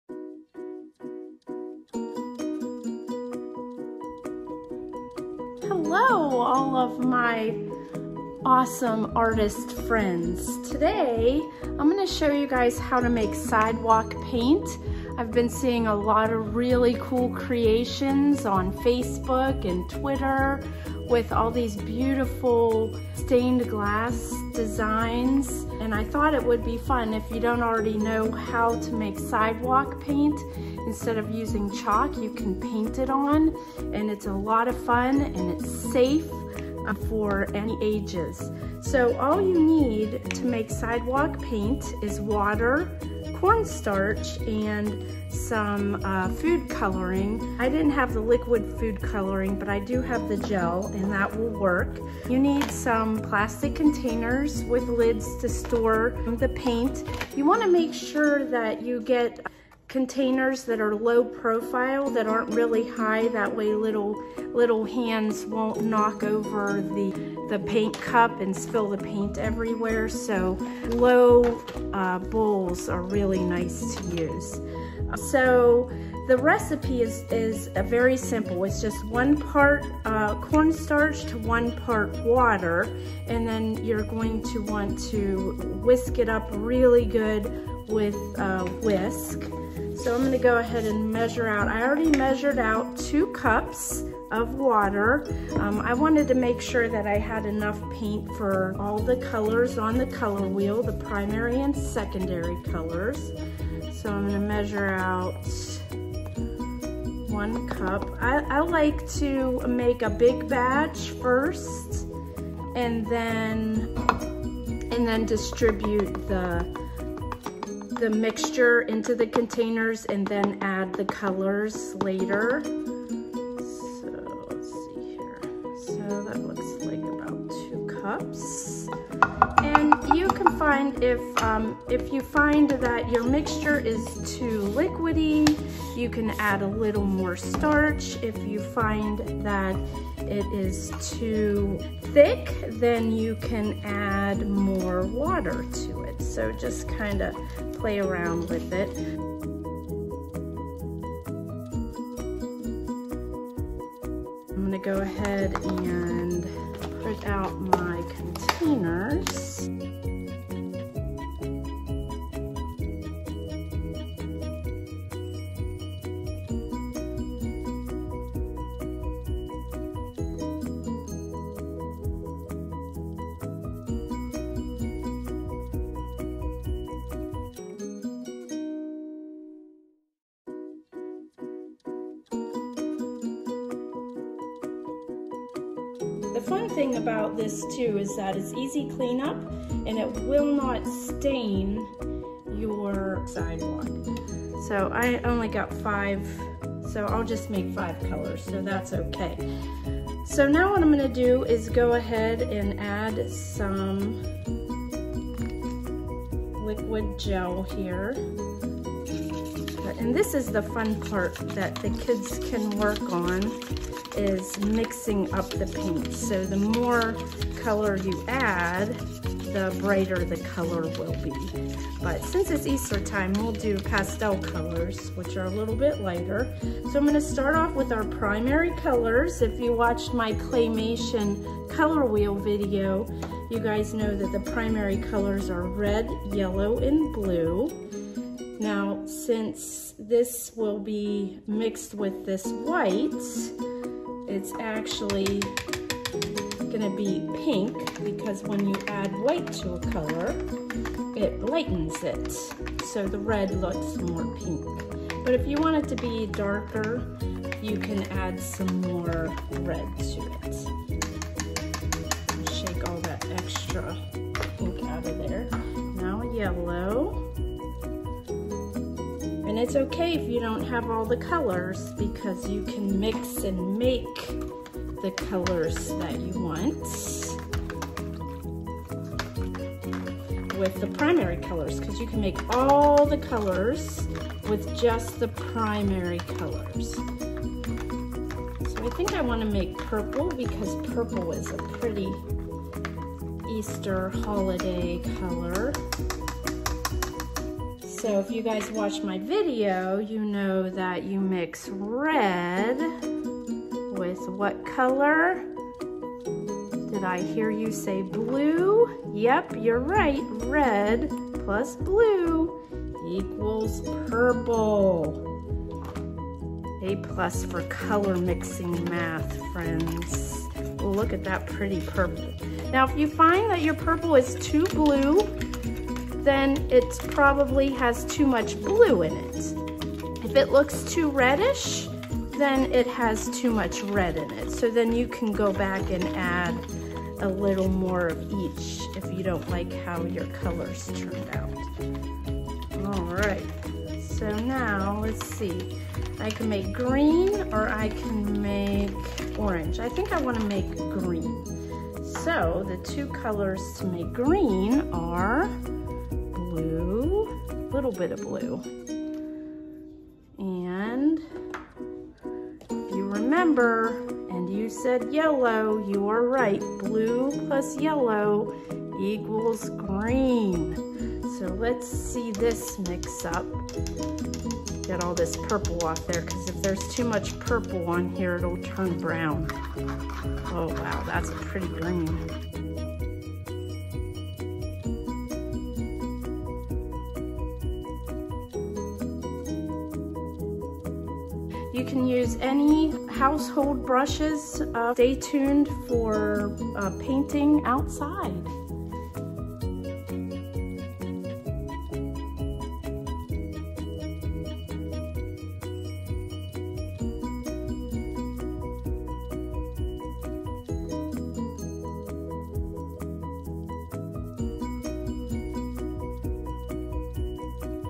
DIY Sidewalk Paint
Make your own sidewalk paint. Safe for any ages and does not stain your sidewalk!